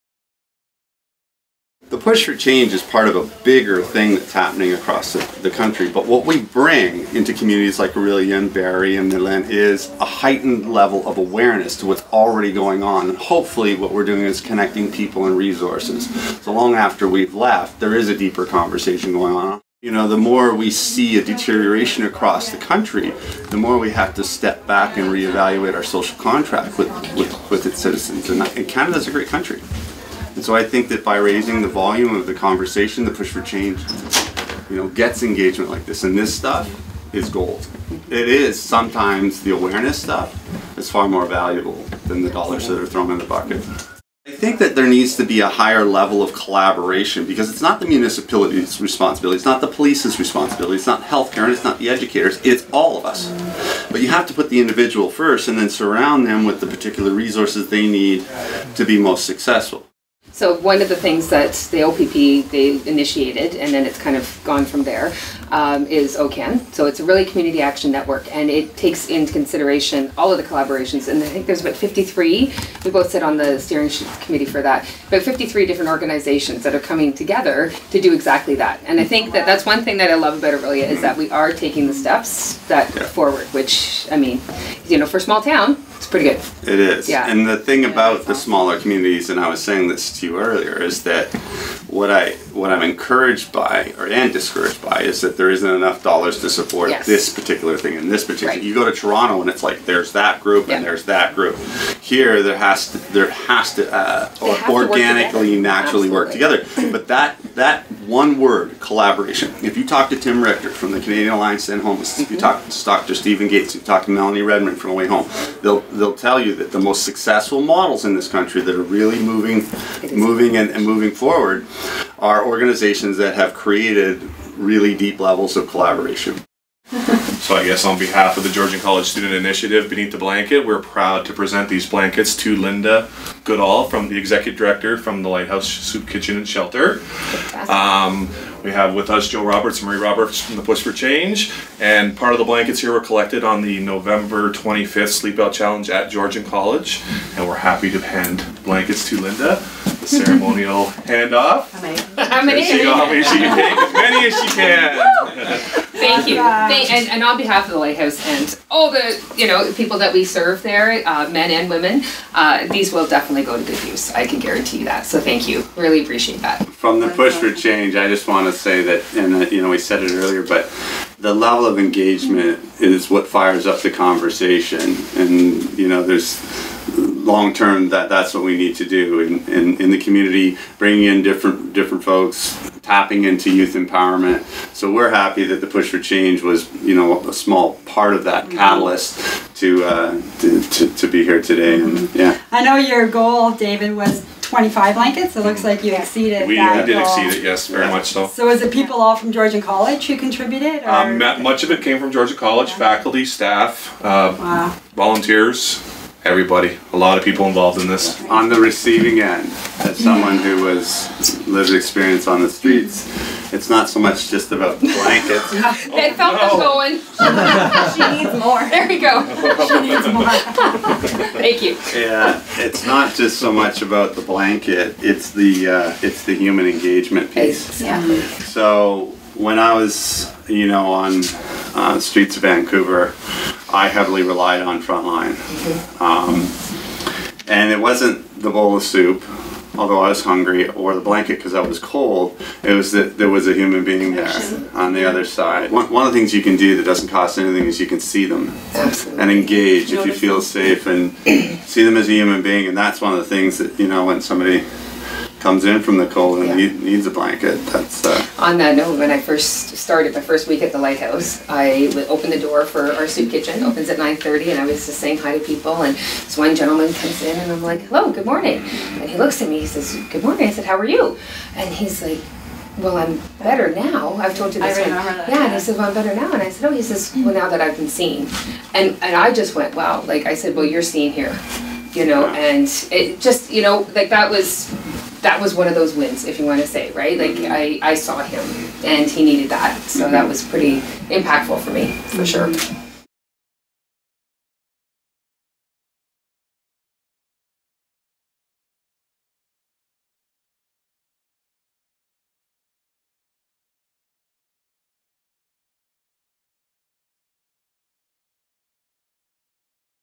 Push For Change at Beneath the Blanket presents blankets to The Lighthouse Soup Kitchen and Shelter
New Years 2017 is bringing new hope to those who must endure the stigma and suffering of being homeless, and our crew at Five Points Media are happy to be on the front lines working with those who know what is needed to help them. Joe Roberts and his wife Marie were in Orillia on January 1st as part of a movement they are spearheading against youth homelessness called THE PUSH FOR CHANGE. Their visit was a well-timed shot in the arm to David James Watt and Michelle White of Beneath the Blanket and also Linda Bushell Goodall, the Executive Director of The Lighthouse Soup Kitchen & Shelter in Orillia. Also on scene was Jill Dunlop of Georgian College, who is also an advocate for youth.